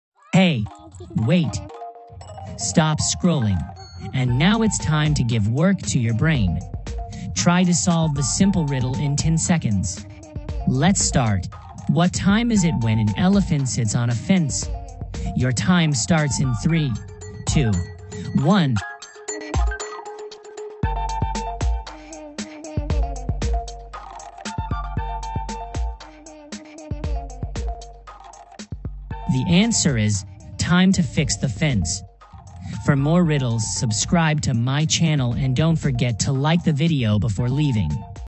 🐘 WHO AM I ?😁 | RIDDLE 001 | KANZ BRAIN BUFFLE
What time is it when an elephant sits on a fence?